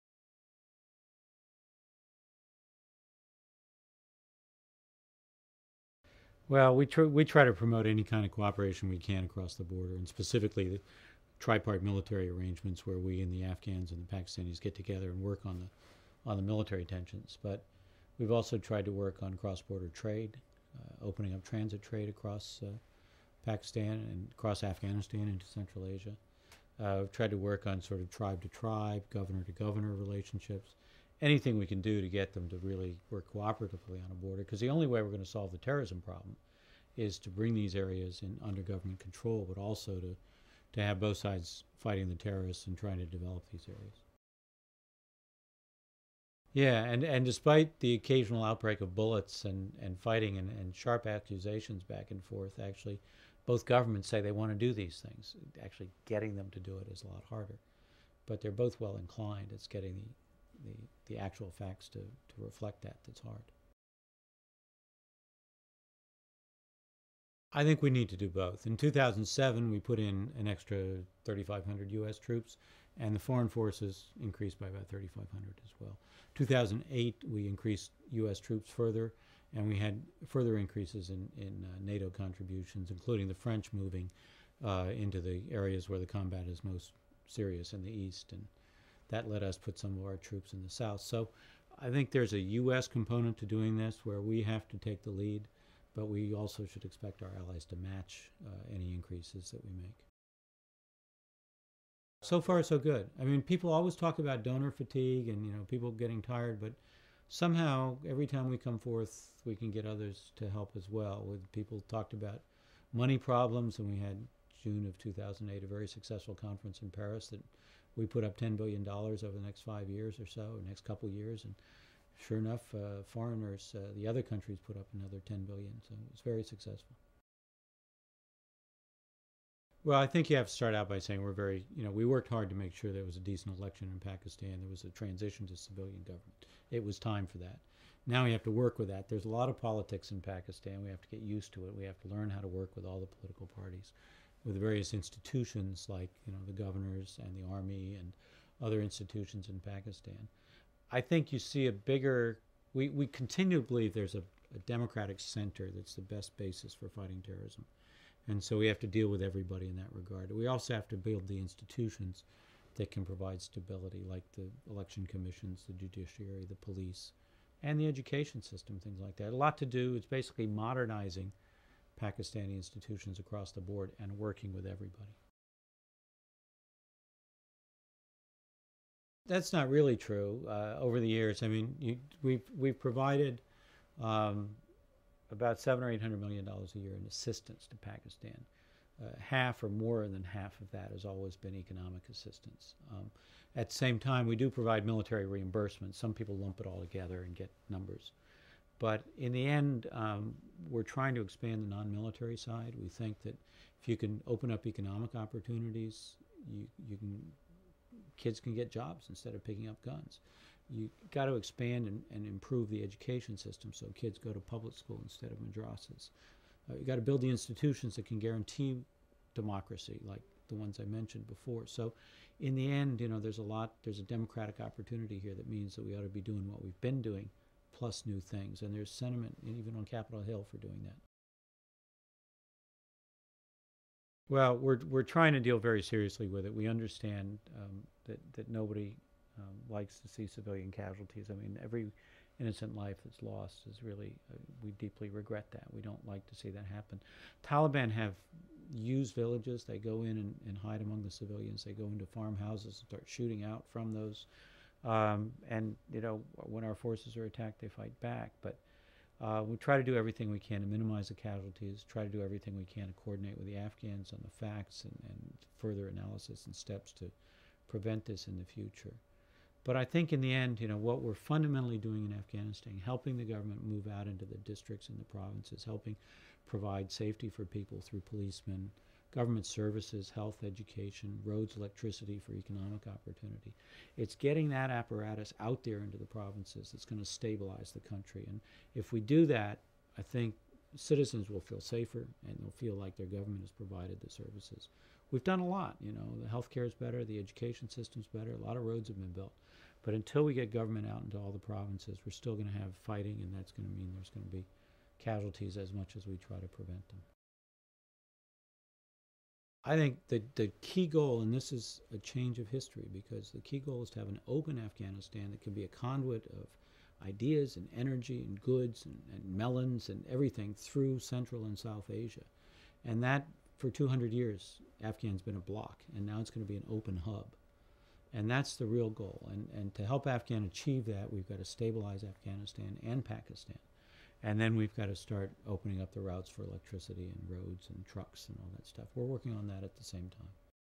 Richard Boucher, GDTV 2009, Extended Interview
Assistant Secretary for Central and South Asian Affairs at  the U.S. Department of State Richard Boucher speaks with GDTV on Pakistan and Afghanistan.